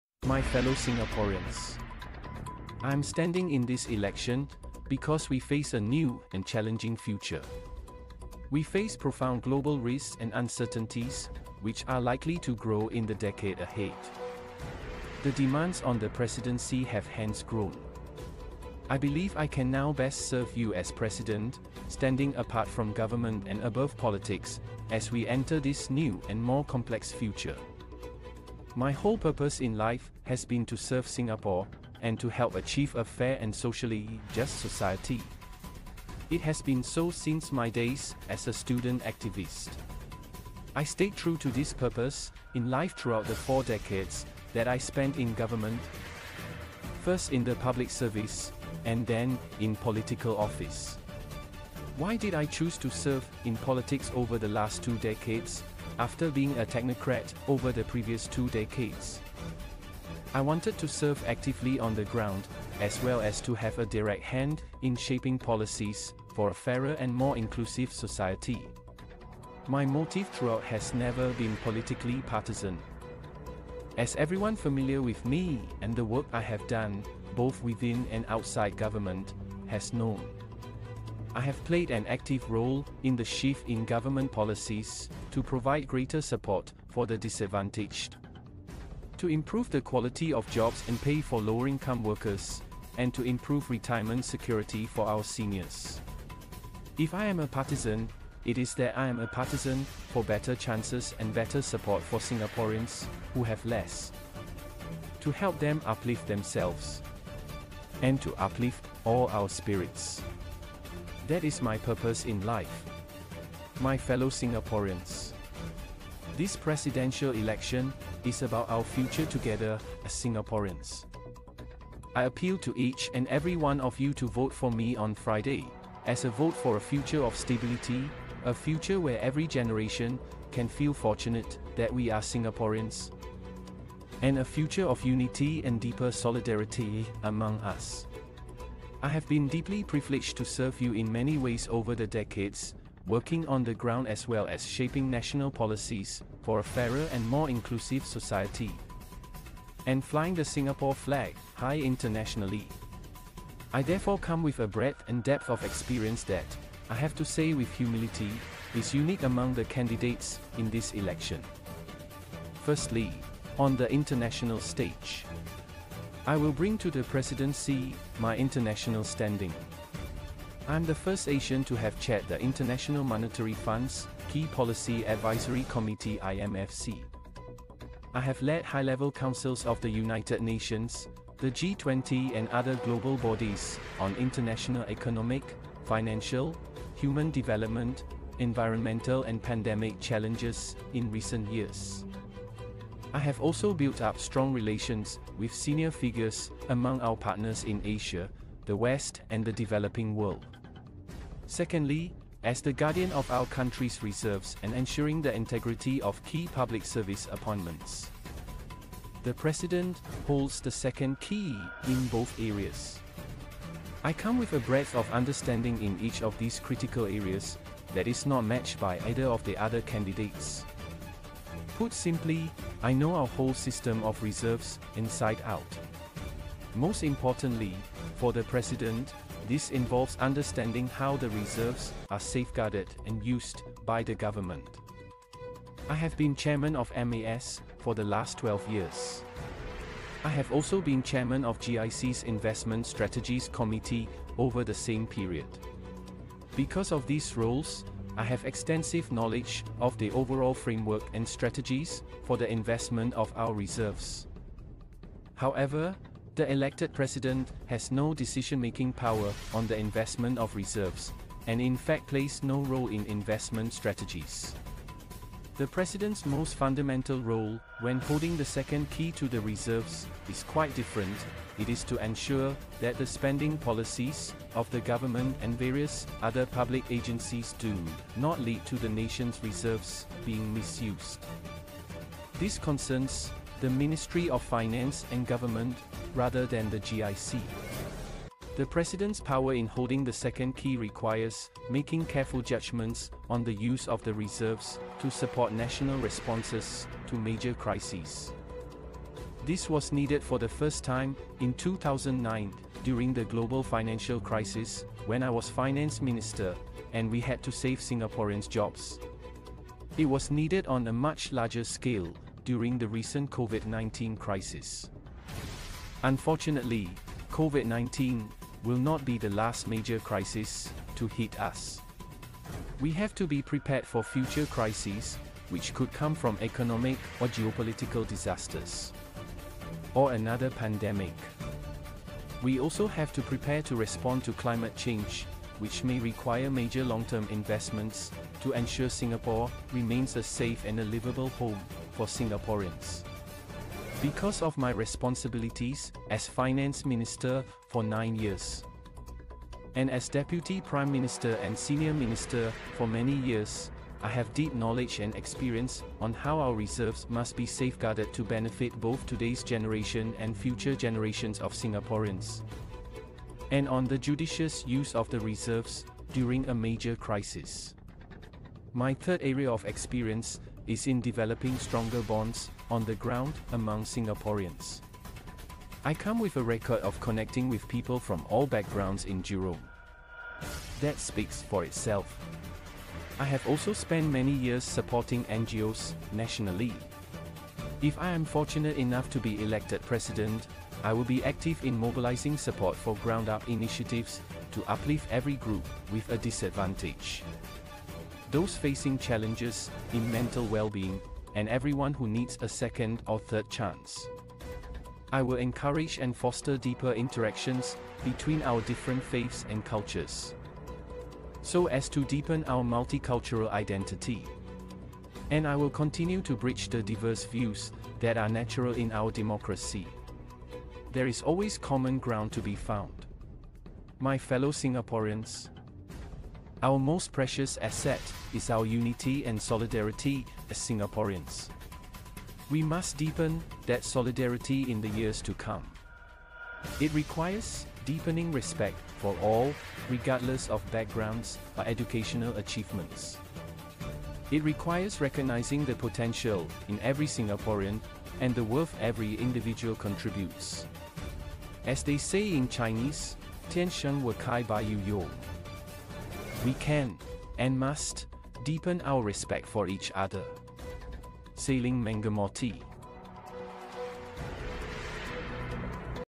Second Presidential Candidate Broadcasts Tharman's Speech in Full
1. The article discusses the broadcast of Tharman Shanmugaratnam's speech in full, who is a potential candidate for the Singapore presidential election.
2. Tharman Shanmugaratnam is a highly respected figure in Singapore and has previously served as the Deputy Prime Minister and Finance Minister.
3. The broadcast of his speech is seen as a significant development in the presidential election campaign, as it indicates his potential candidacy.
4. Tharman Shanmugaratnam's speech focuses on the need for unity, inclusiveness, and a sense of common purpose in Singapore.
5. He emphasizes the importance of maintaining Singapore's core values, such as meritocracy and multiculturalism, while addressing the challenges and changes faced by the country.
6. Tharman Shanmugaratnam's speech also highlights the need to address economic inequality and ensure that all Singaporeans benefit from the country's progress.